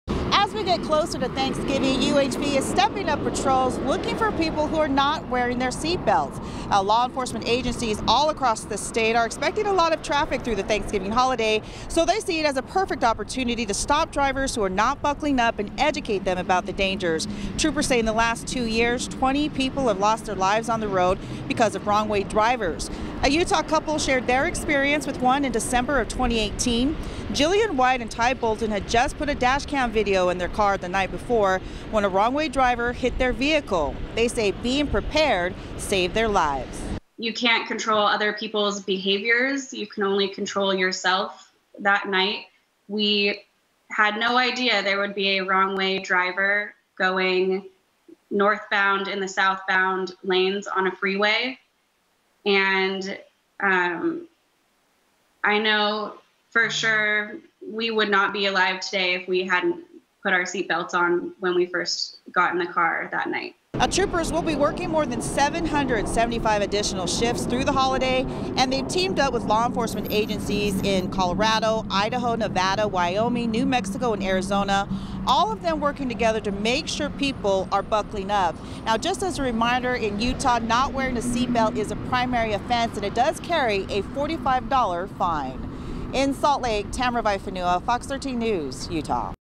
UHP troopers are aggressively enforcing the seat belt law
UHP troopers are aggressively enforcing Utah's seat belt law.